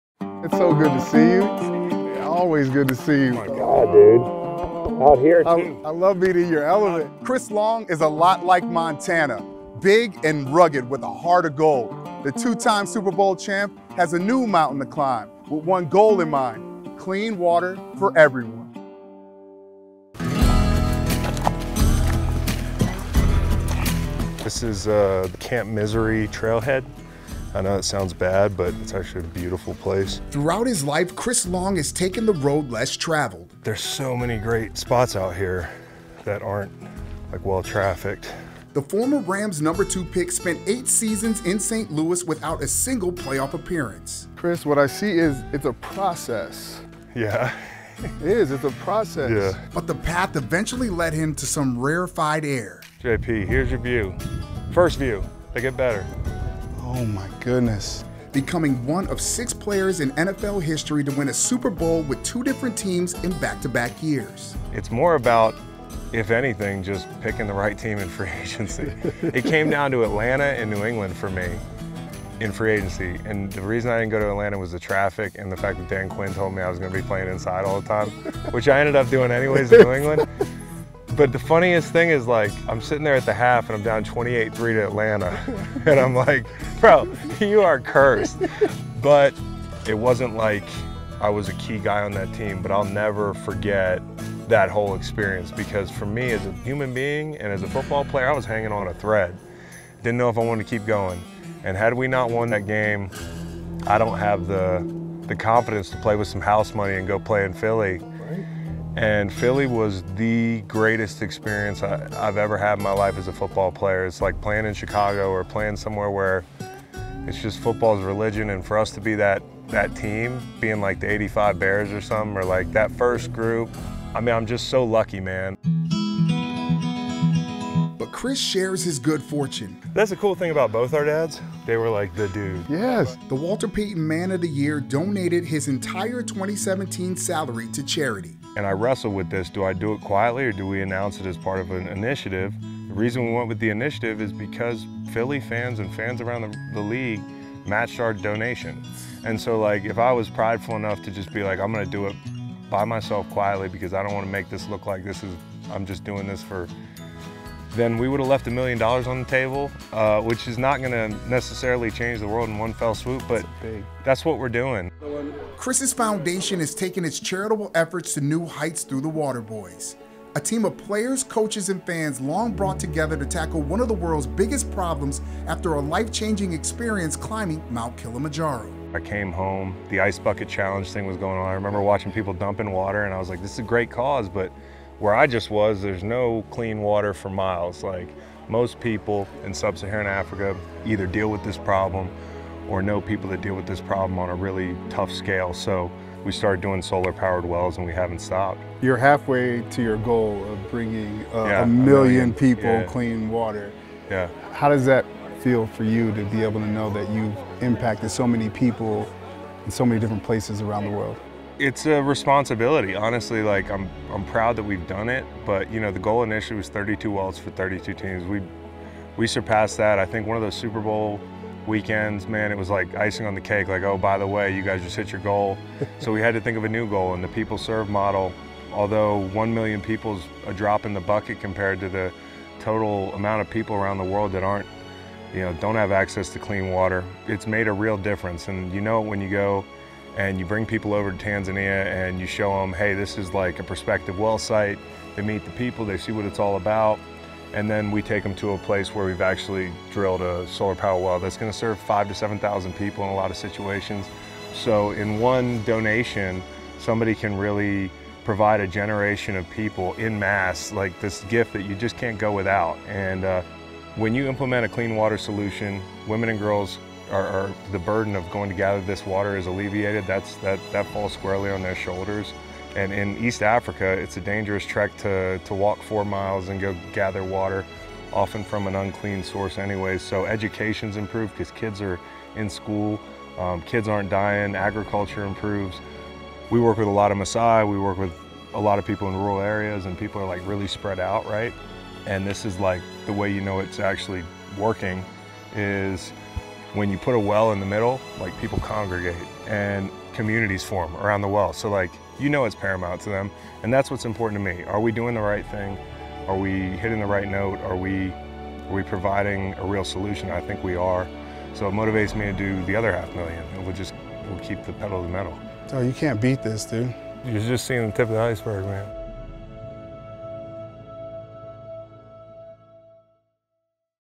Chris Long continues quest to give clean water to everyone
After climbing Mt. Kilimanjaro, Chris Long was inspired to repay the people of Tanzania with clean water. Now, the two-time Super Bowl champ is taking his charitable efforts to new heights, building wells around the world